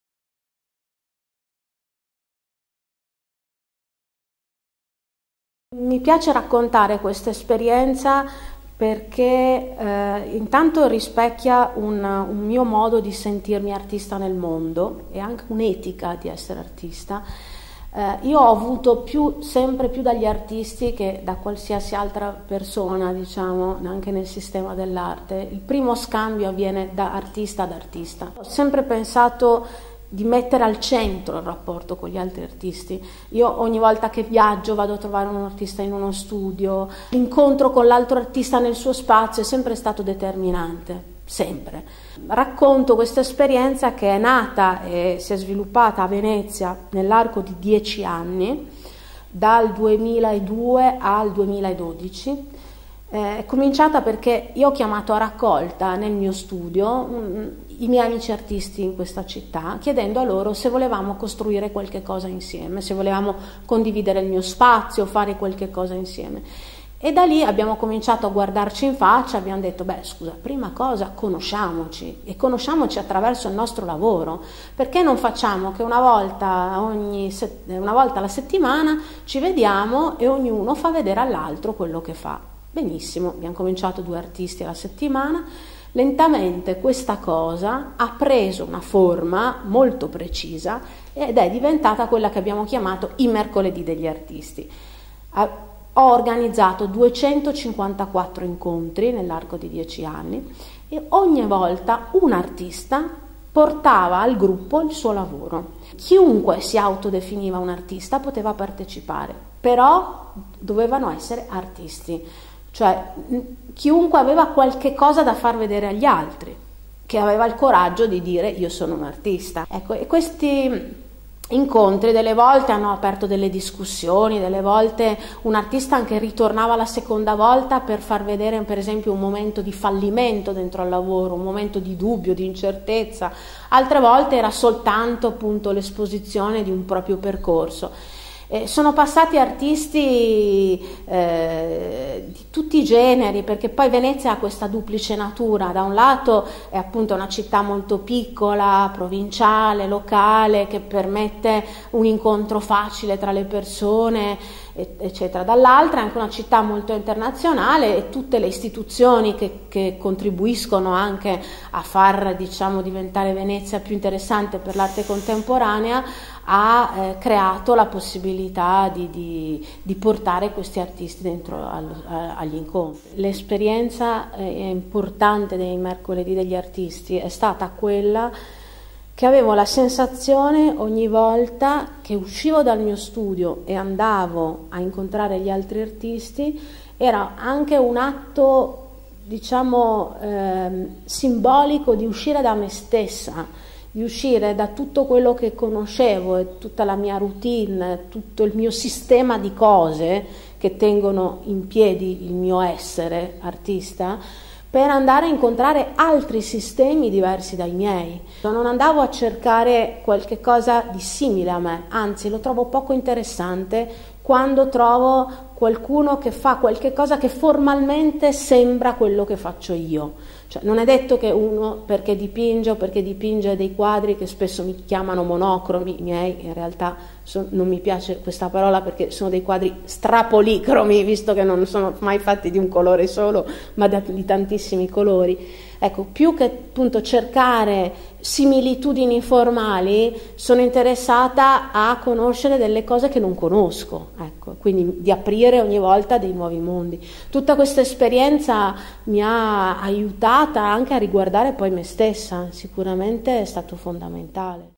Maria Morganti e i mercoledi degli artisti
Maria Morganti racconta di una iniziativa da lei organizzata a Venezia, "I mercoledì degli artisti", incontri nei vari studi con i diversi artisti per confrontarsi sulle modalità del fare arte.